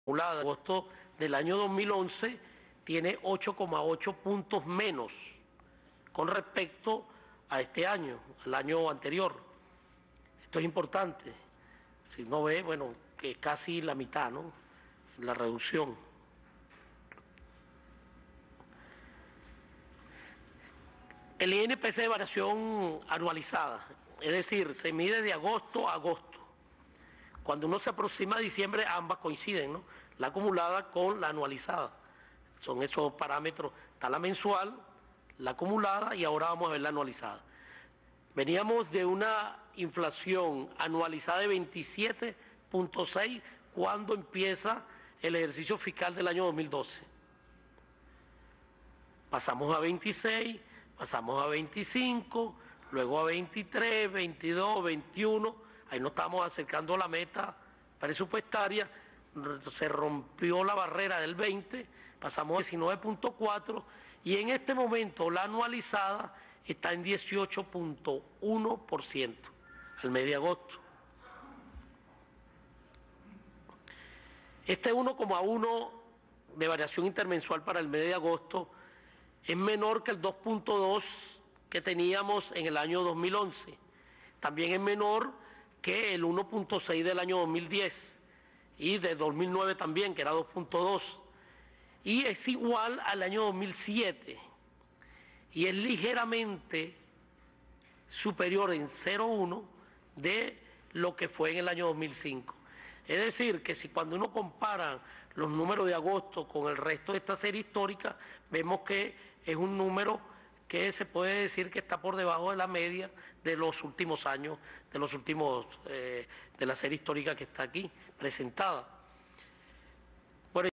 BCV: Inflación de agosto se ubicó en 1 % (PRIMICIA Primero y Mejor)
Captura de pantalla de VTV